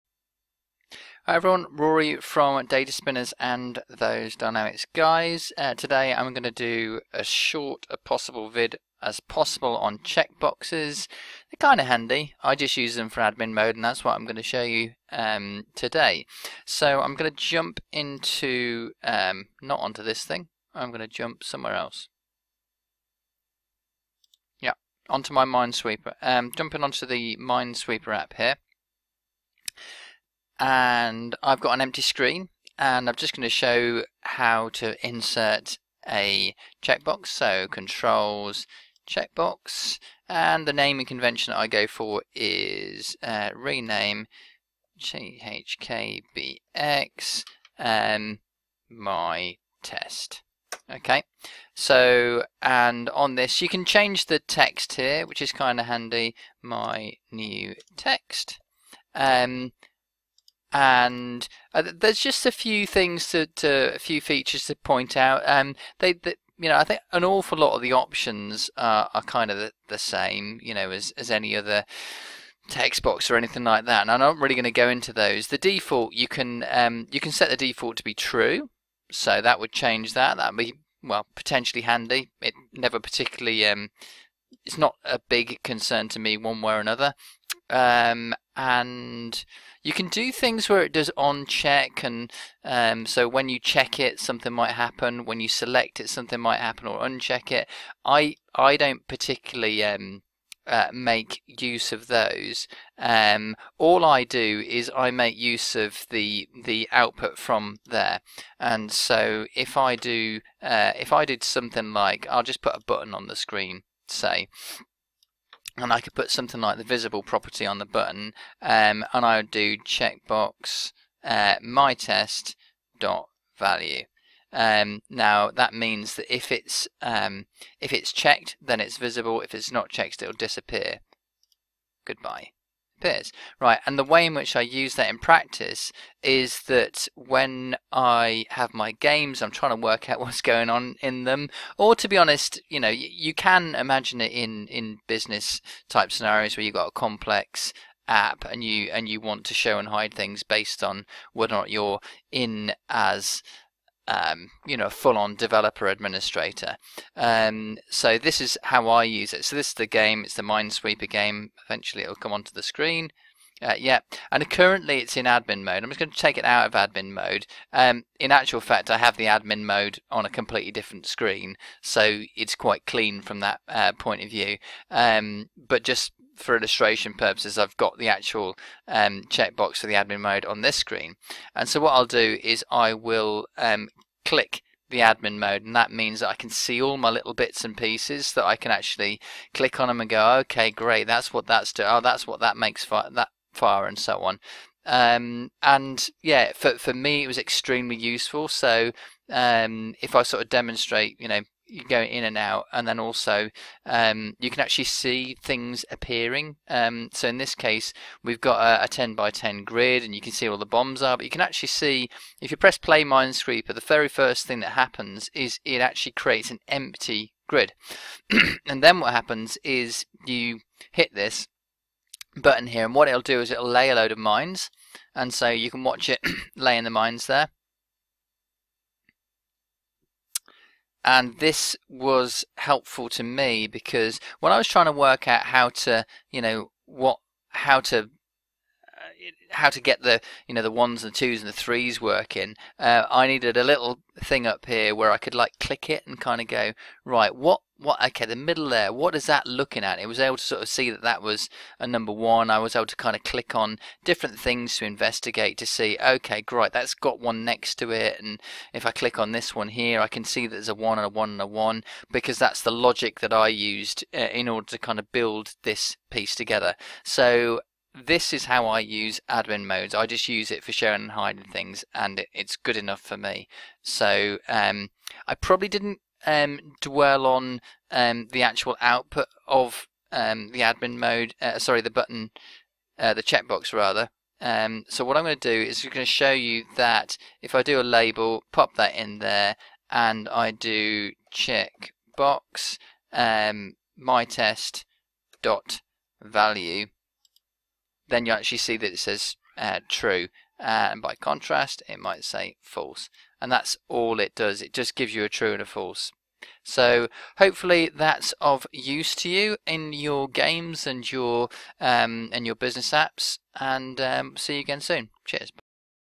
PowerApps Tuesday Tutorials #18 Using Checkboxes
In this short vid I show how I make use of checkboxes in my apps.
The vid isn't meant to be utterly comprehensive, just enough to make you see that from time to time it's worth using a checkbox!